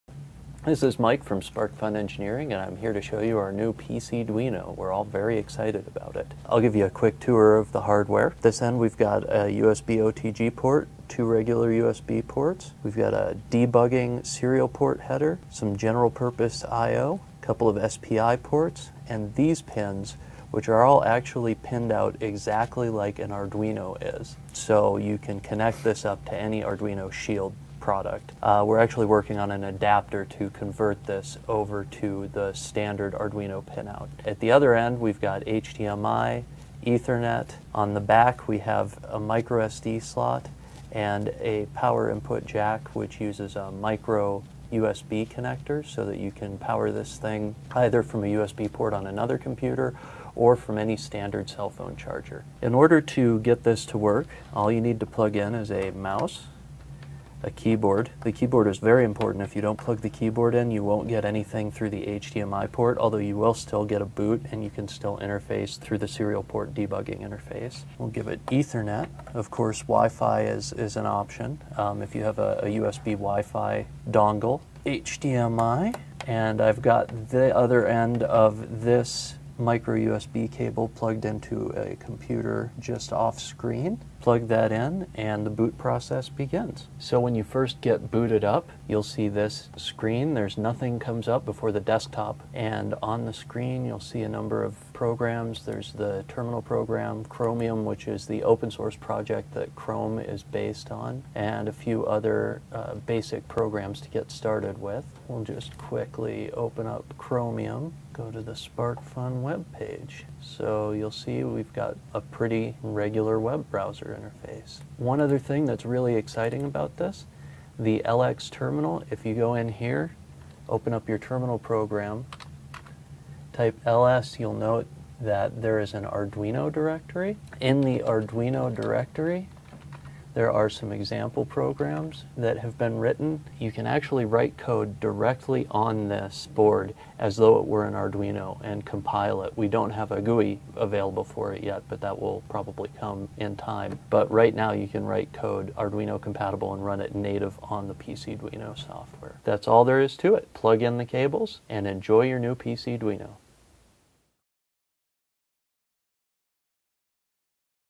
SparkFun pcDuino QuickStart Guide
The pcDuino is a high performance, cost effective mini PC platform that runs full-featured operating systems such as Ubuntu and Android ICS.